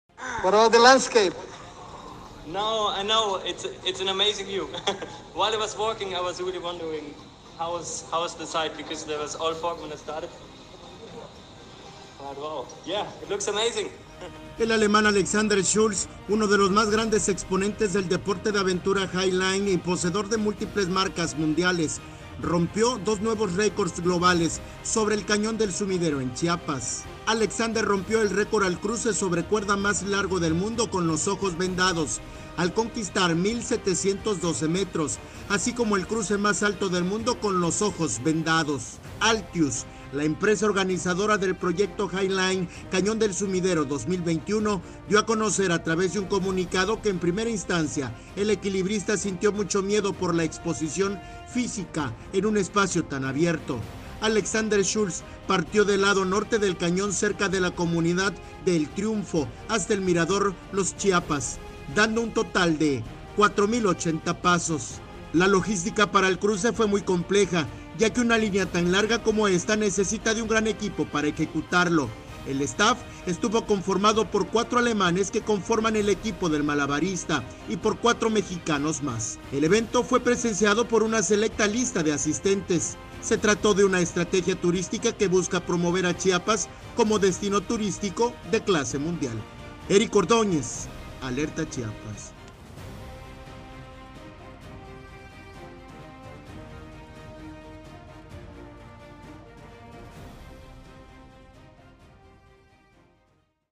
Sobre la cuerda floja, promoción turística de Chiapas
El alemán, ALEXANDER SCHULZ, uno de los más grandes exponentes del deporte de aventura HIGHLINE y poseedor de múltiples marcas mundiales, rompió dos nuevos récords globales sobre el Cañón del Sumidero en Chiapas.

Se trató de una estrategia turística que busca promover al Estado de Chiapas como destino turístico de clase mundial.